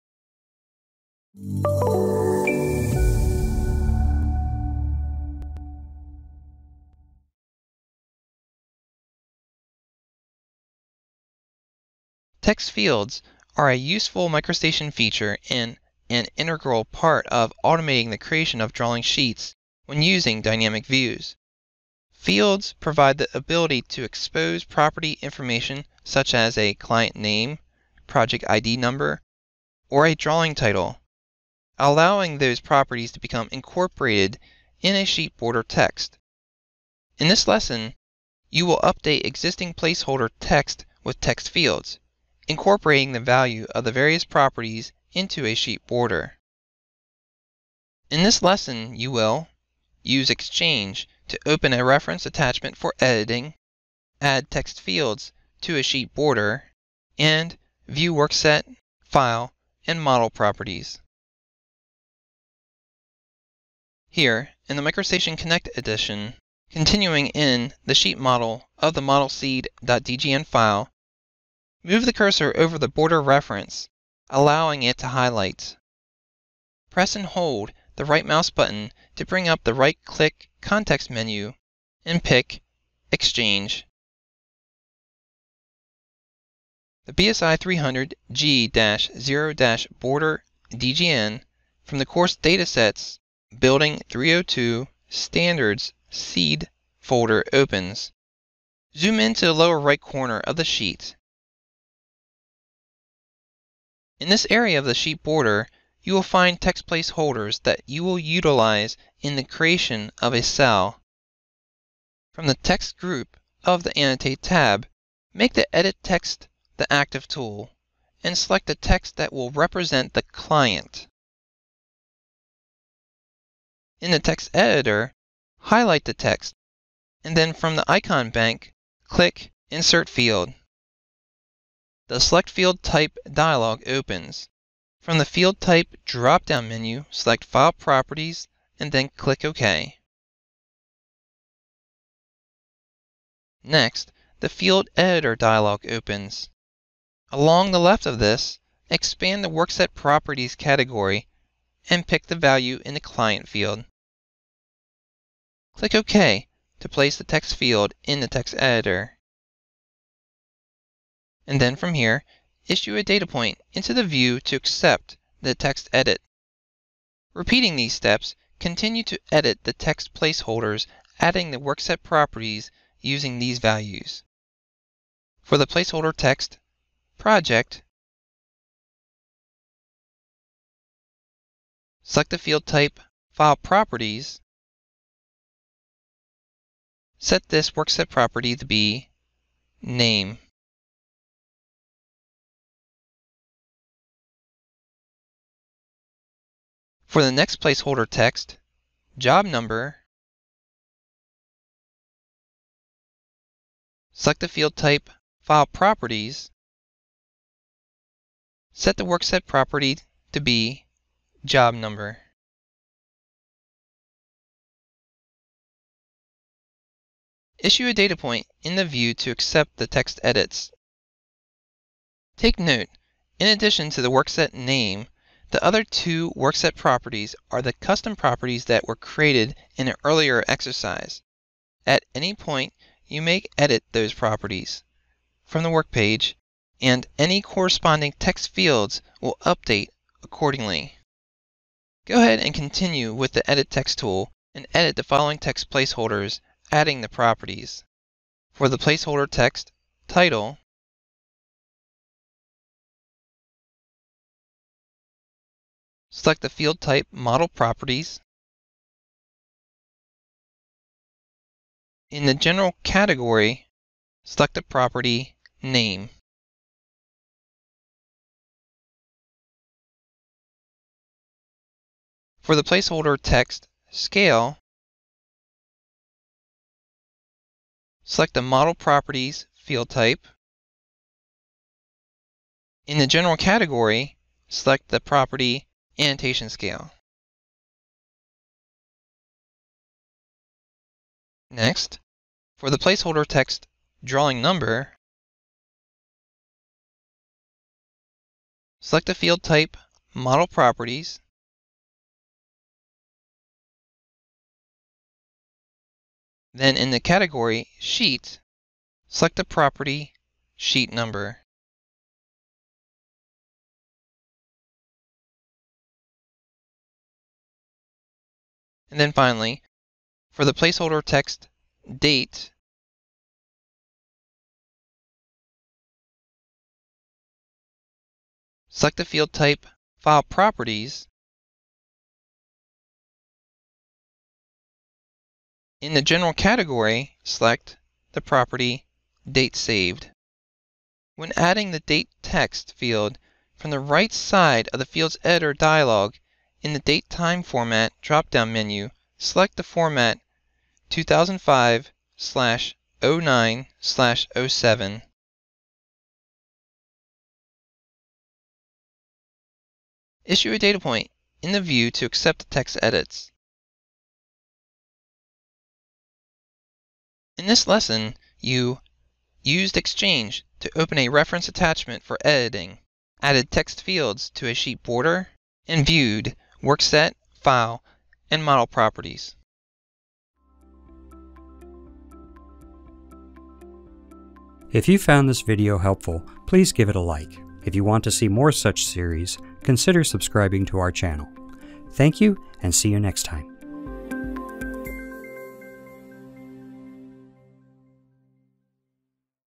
10 Defining a Custom Title Block with Text Fields
Text Fields are a useful MicroStation feature and an integral part of automating the creation of drawing sheets when using Dynamic Views. Fields provide the ability to expose property information such as a client name, project ID number, or a drawing title, allowing those properties to be incorporated in sheet border text. In this video, you will update existing "placeholder" text with text Fields, incorporating the value of various Properties into a sheet border.